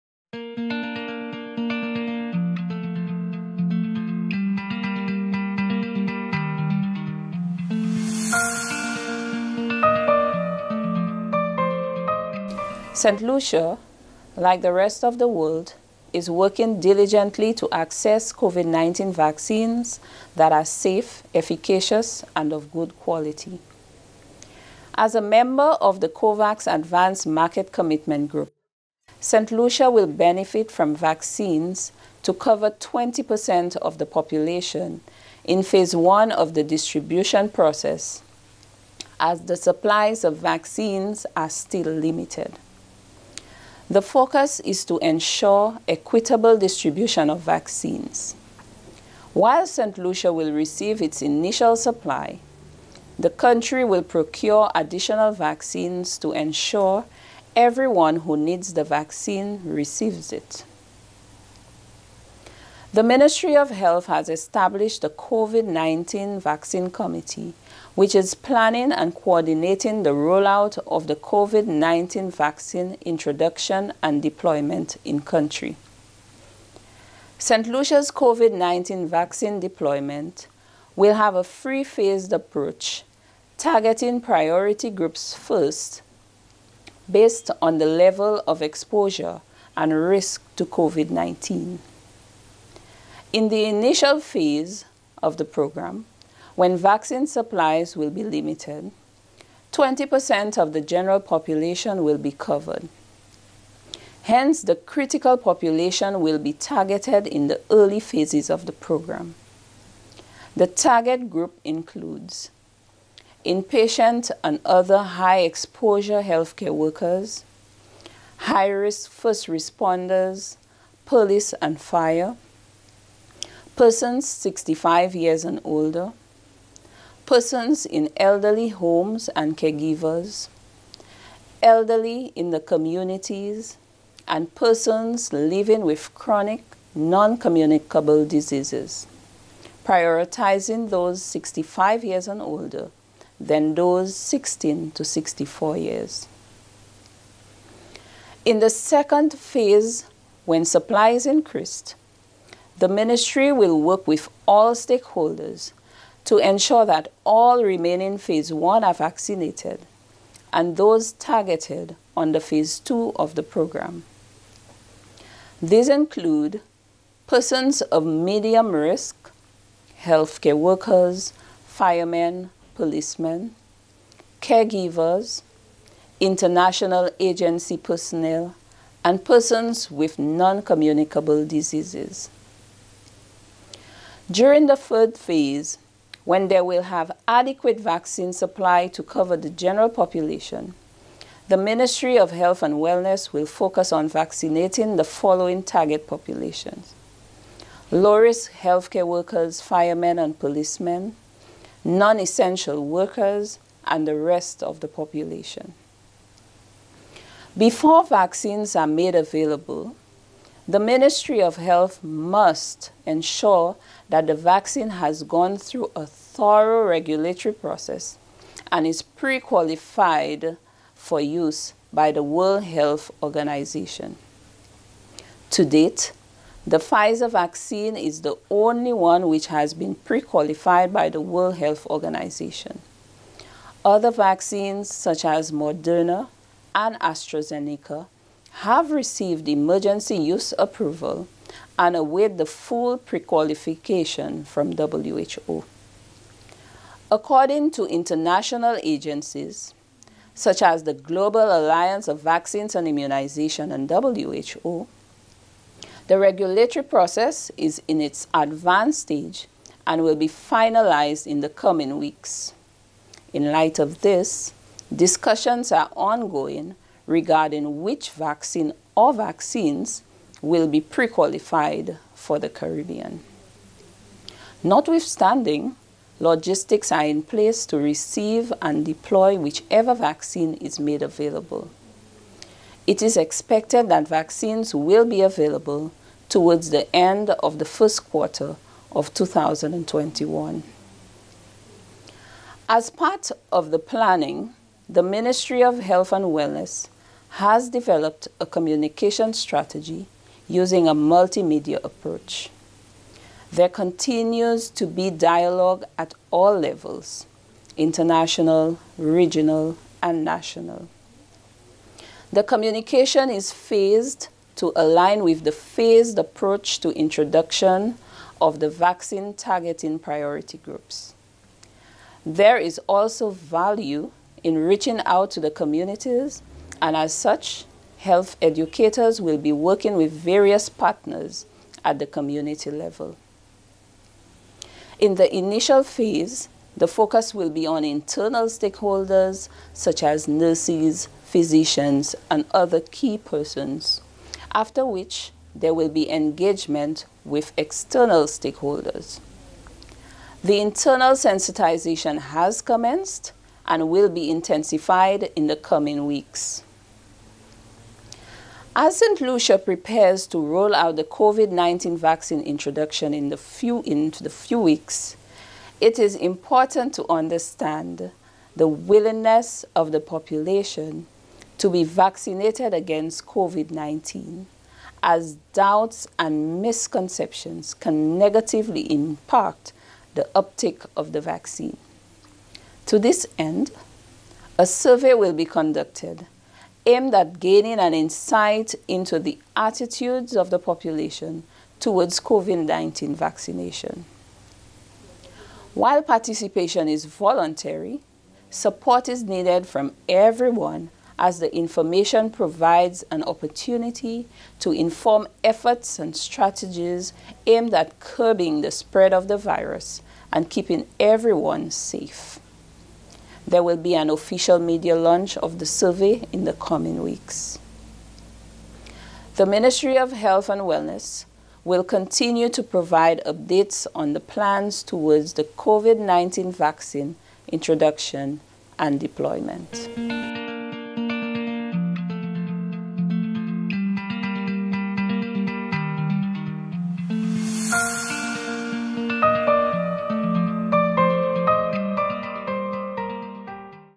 Update on COVID-19 vaccine for Saint Lucia
As a member of the COVAX Advanced market Commitment (AMC) group, Saint Lucia will benefit from vaccines to cover 20 percent of the population in Phase 1 of the distribution process, as the supply of vaccines are still limited. The focus is to ensure equitable distribution of
vaccines.
While Saint Lucia will receive its initial supply, the country will procure additional vaccines to ensure everyone who needs the vaccine receives it.